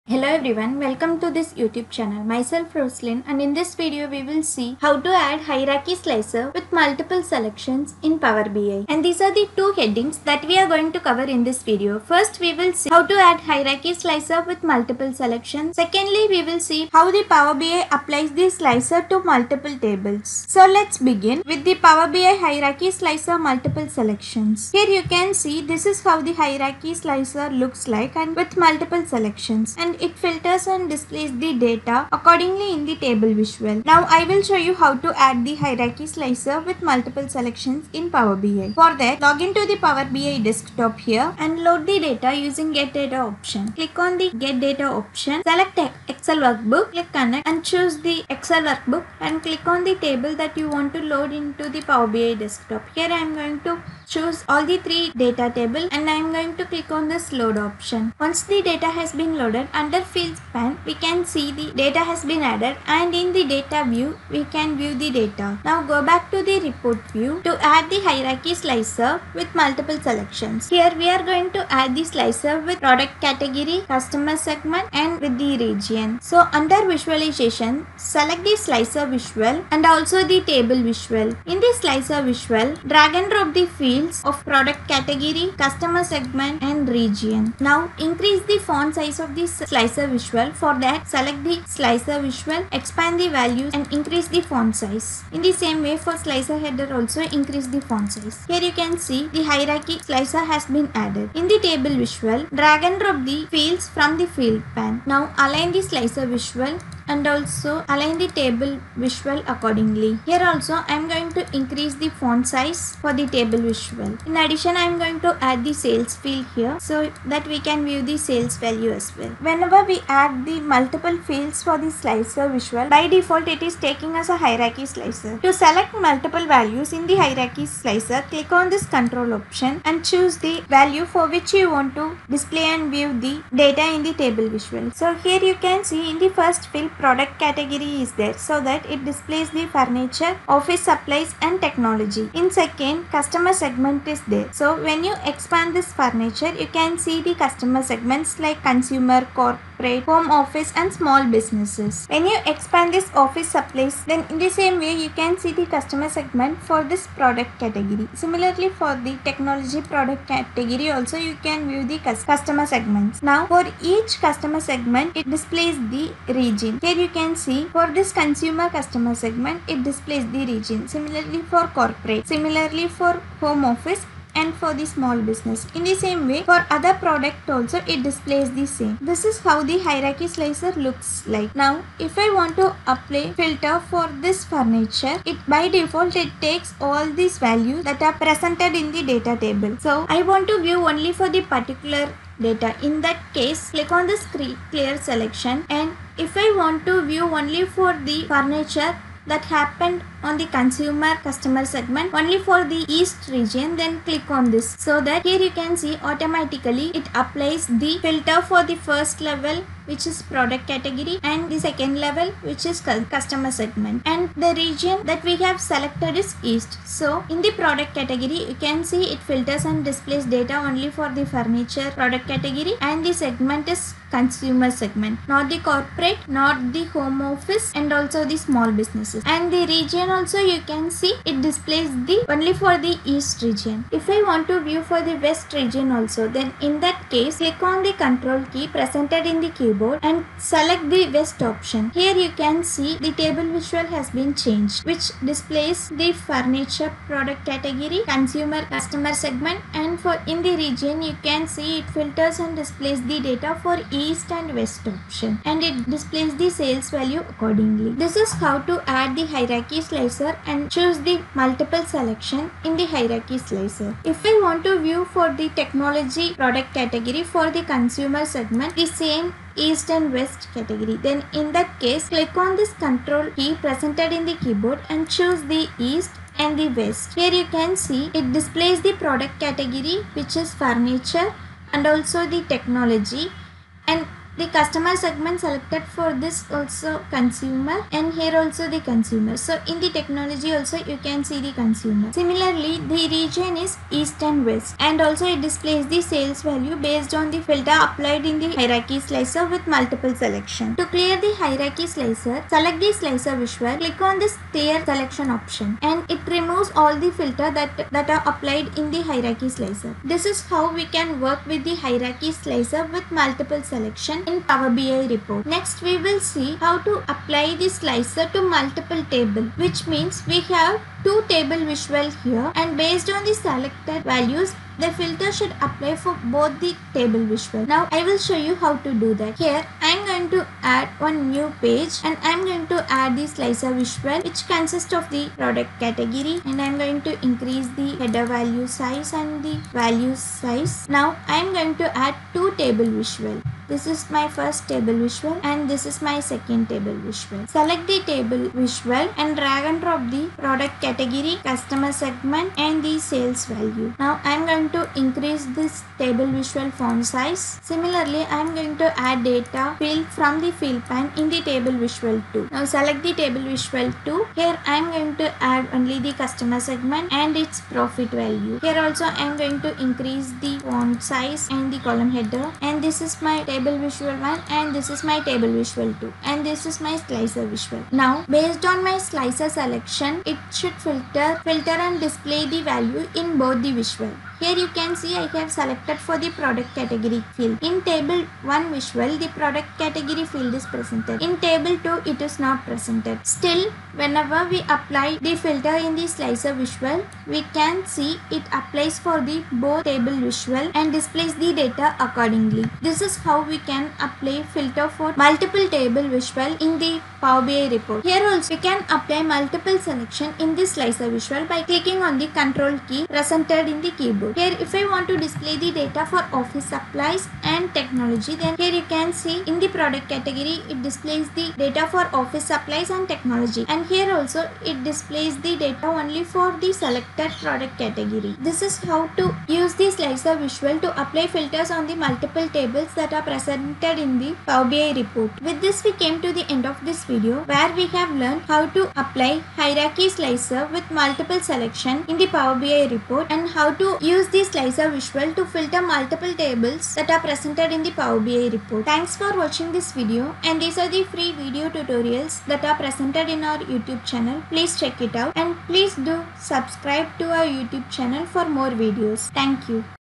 Power BI - Hierarchy Slicer | Create Hierarchy Slicer in Power BI | Hierarchy Slicers in Power BI
In this Power BI video, I have shown you how to create a Hierarchy Slicer in Power BI Desktop.
By default, whenever we add multiple fields to the slicer visual, it will be considered a Hierarchy slicer, I have also shown you how we can select Multiple values in the Power BI Hierarchy slicer.
Additionally, I have shown you how to apply a slicer filter to multiple tables in the Power BI report.